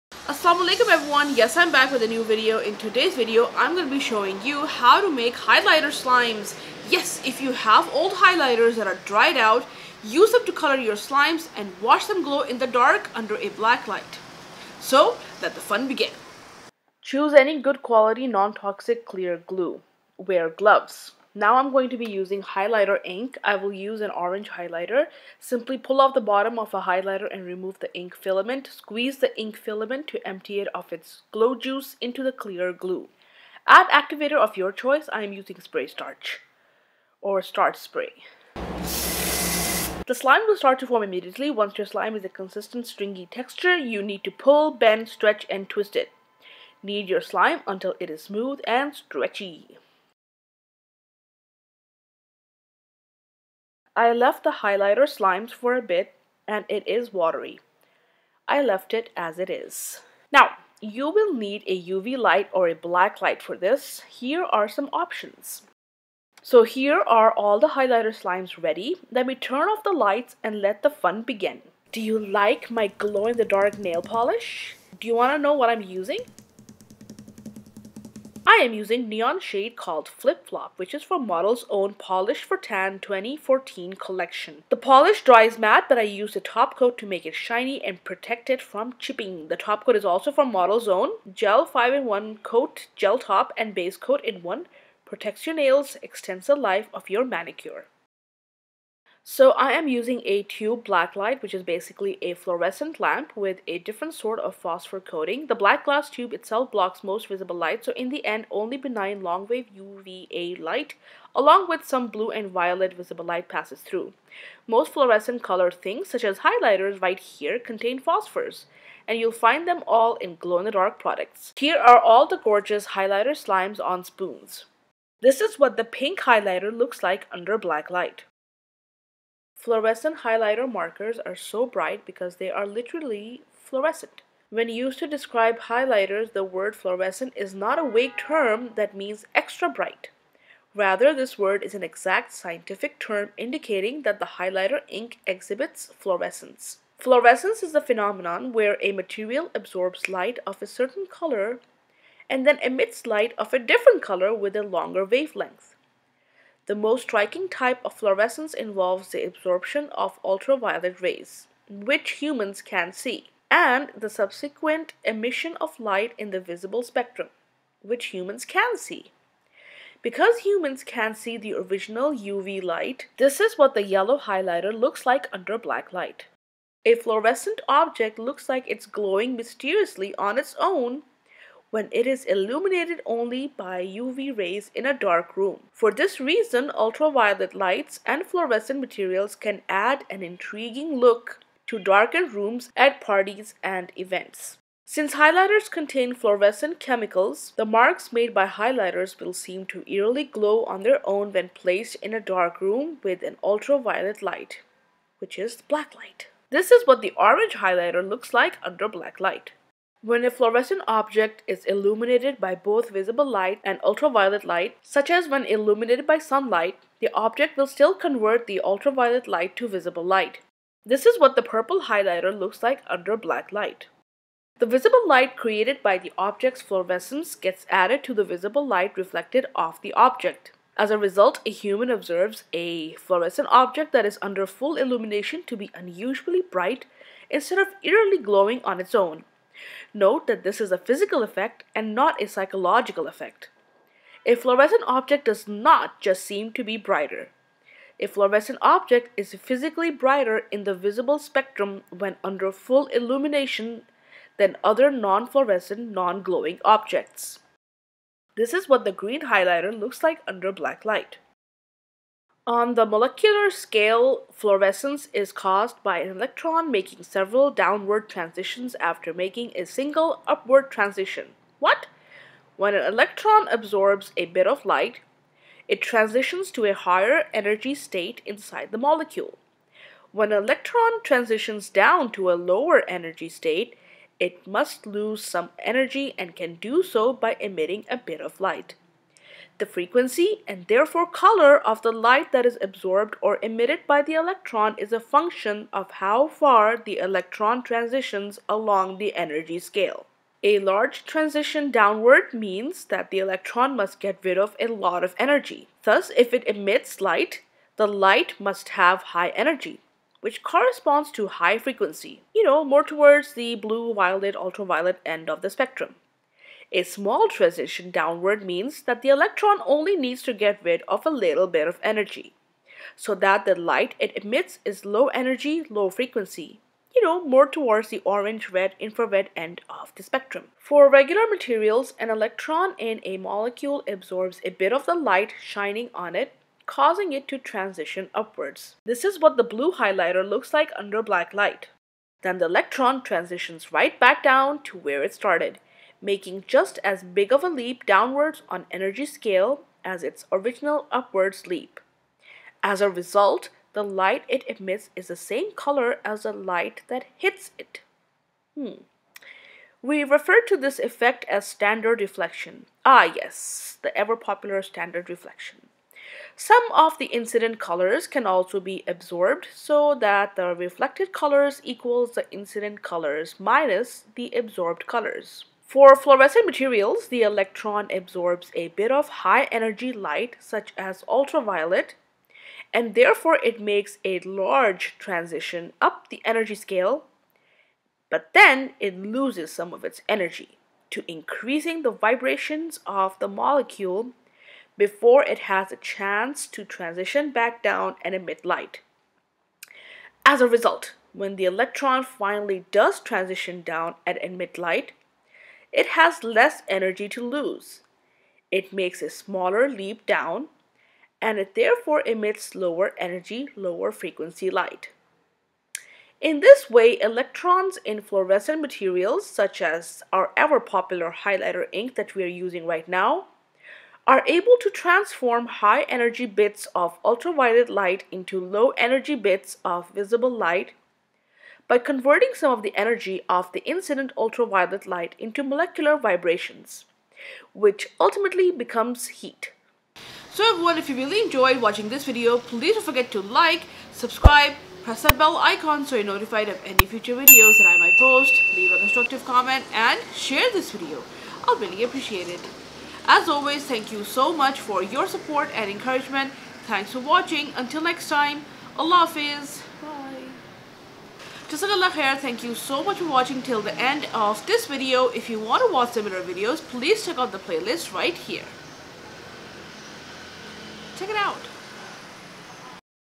HOW TO MAKE GLOW IN THE DARK HIGHLIGHTER SLIMES!
Glowing Slimes using Highlighters!
DIY GLOW IN THE DARK SLIME!/ How to make Glowing Slime with highlighters!!!
Stretching glowing highlighter slimes!
Highlighter slime play under black light!